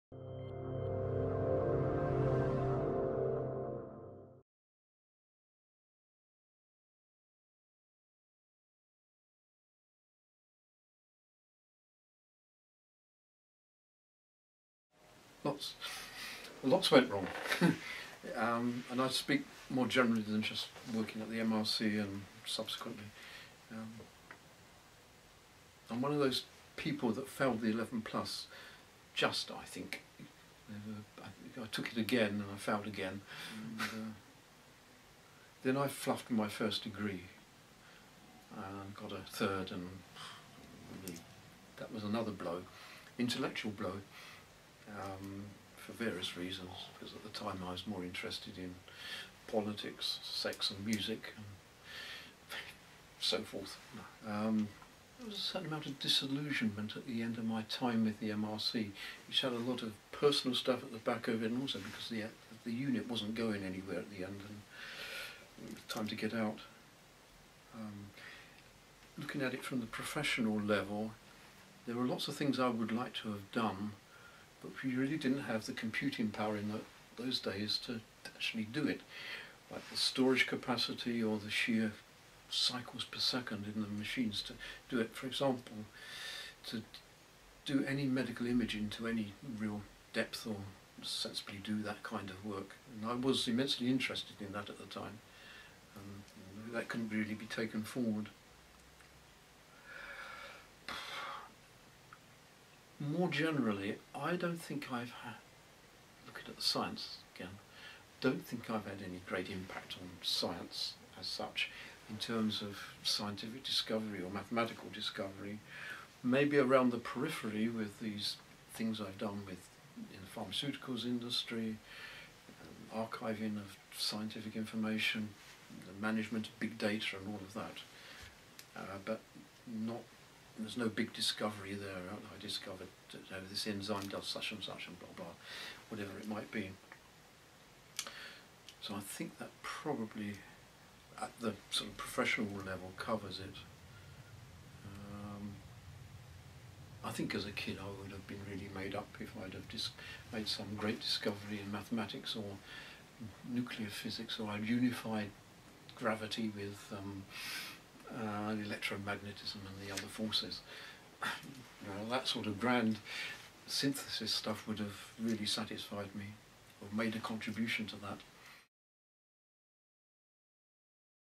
Lord, Philip 04 Setbacks
Interview with Mr Philip Lord, conducted by Mr Adam Wilkinson, for the History of Modern Biomedicine Research Group, 21 July 2016, in the School of History, Queen Mary University of London. Transcribed by Mrs Debra Gee, and edited by Professor Tilli Tansey.

Mr Philip Lord MSc CMath FRSA (b. 1945) studied mathematics at Reading and London Universities, and also has a teaching certificate from the University of Sussex. He was a member of the MRC Air Pollution Unit’s scientific staff between 1968 and 1978, after a brief spell teaching. In the MRC, he undertook research applying mathematics and computer techniques to the study of lung function, lung morphology, and respiratory flow dynamics. The research involved him in the development of techniques for the automation of lung function measurement. He went on to a post as Technical Manager for medical publishing at Elsevier Science Publishers in Amsterdam, where he later became closely involved in the development of new technologies for scientific publishing. Here he became Vice Chairman of the ISO and NISO committees, which determined the format standards for CD-ROM (ISO9660). In 1991 he joined the pharmaceuticals industry, first at SmithKline Beecham and then GlaxoSmithKline, in which companies he led projects for managing large-scale regulatory documentation and for archiving scientific data. As a leader in the developing science of digital archiving, he set up his own digital archiving consultancy in 2002, and worked internationally to promote best practice. He is now semi-retired, but still teaches digital archiving at the University of Dundee. He was elected a Fellow of the Royal Society of Arts (FRSA) in recognition of his contribution to archiving digital information.

How to cite: Wilkinson A (intvr); Yabsley A (prod) (2017) Lord, Philip: 04 - Setbacks (21-Jul-2016). History of Modern Biomedicine Interviews (Digital Collection), item e2017185. London: Queen Mary University of London.